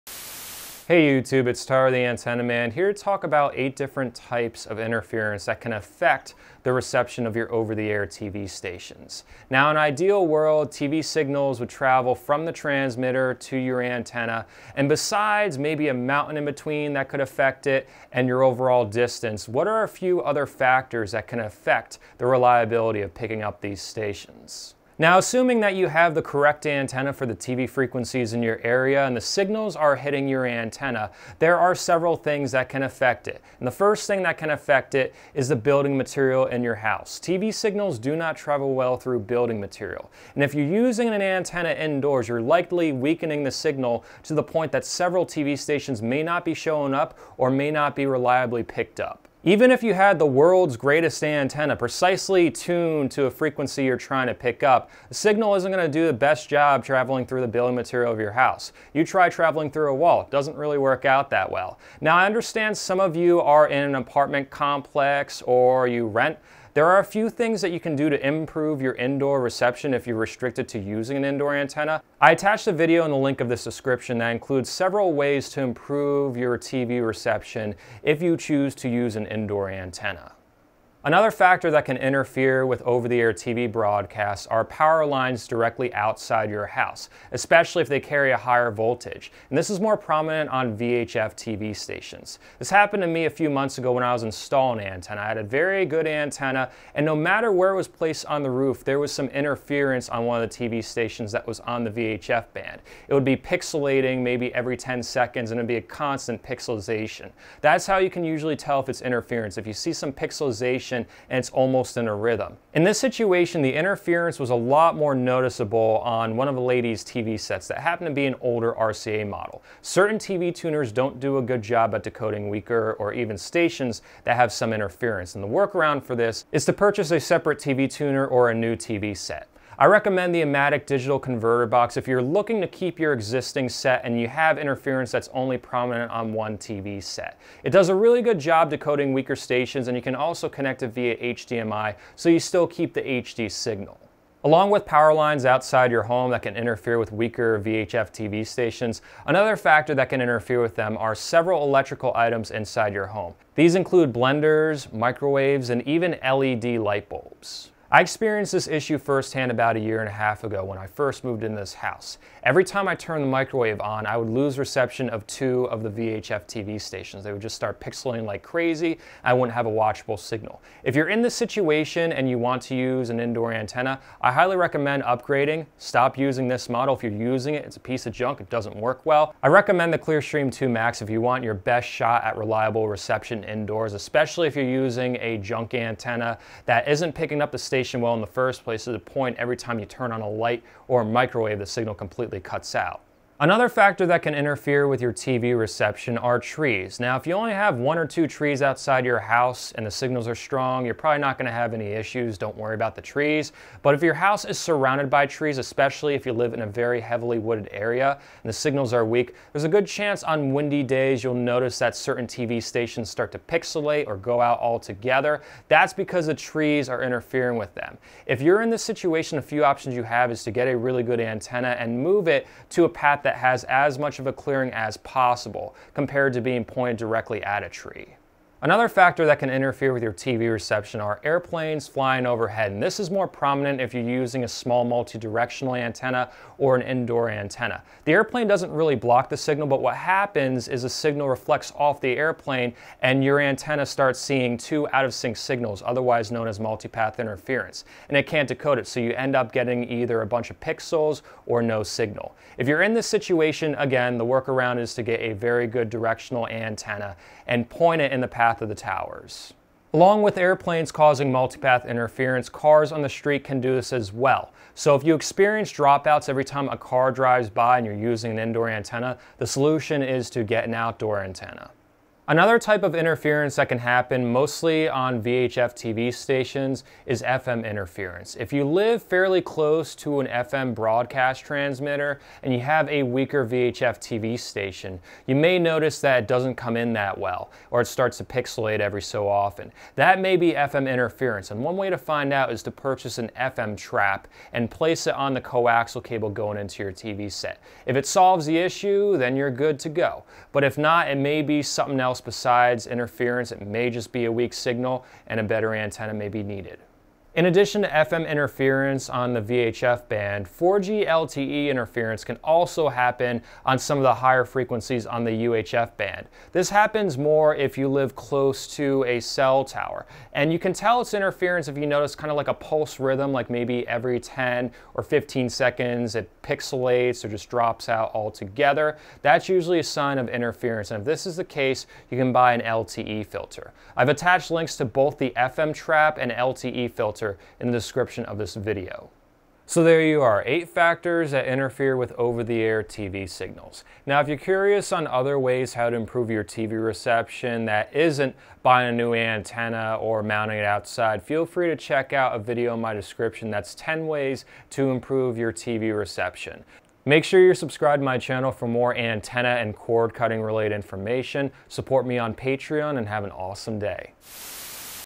Types of Interference That Affects OTA TV Antenna Reception
This video highlights eight different interference factors that can affect your over the air HD TV reception if you use an antenna. Most of these factors are more prominent when using an indoor antenna versus using a better outdoor antenna. Understanding these factors can help improve your TV reception.

Link to Ematic DTV Tuner